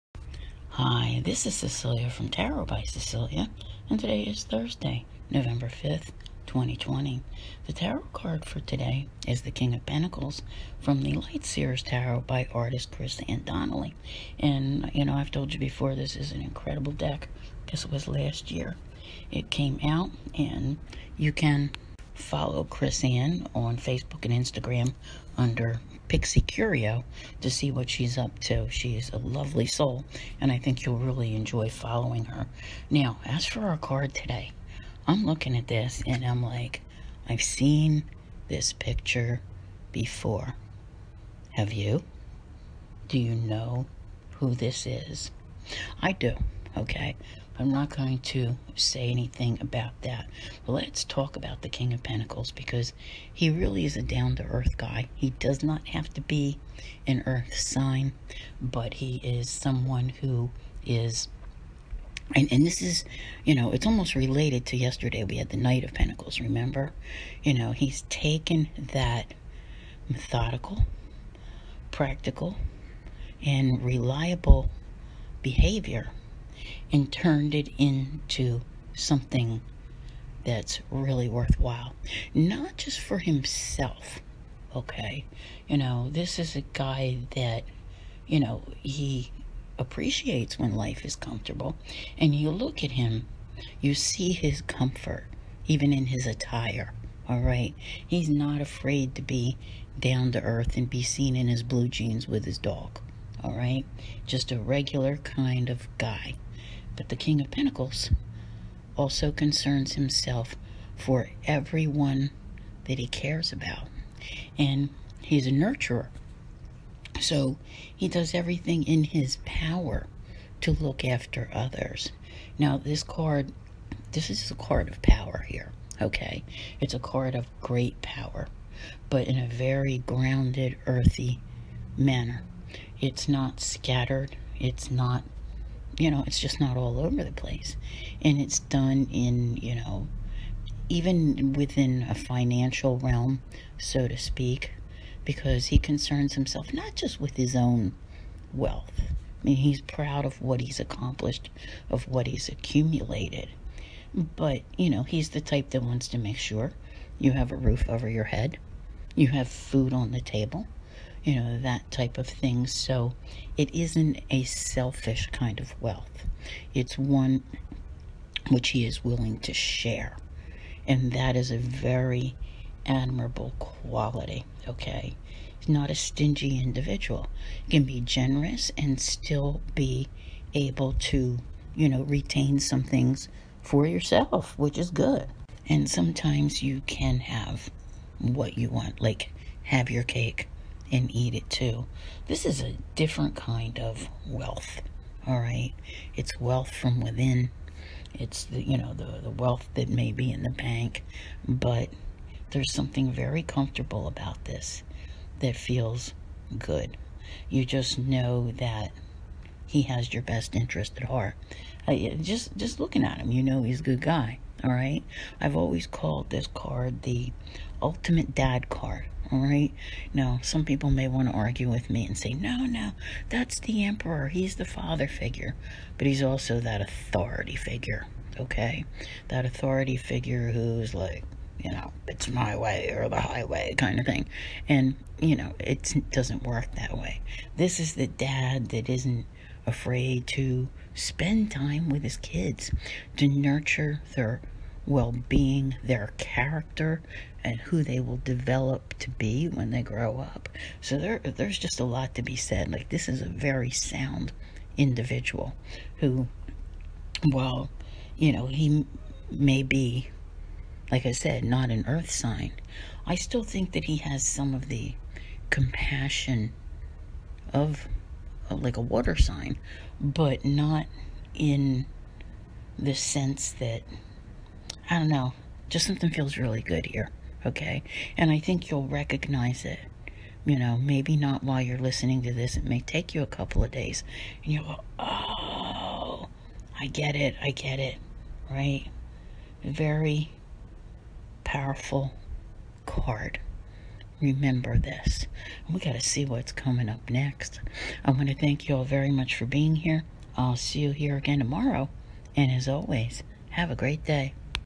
November 5, 2020 - Tarot Card of the Day
King of Pentacles - Thursday, November 5, 2020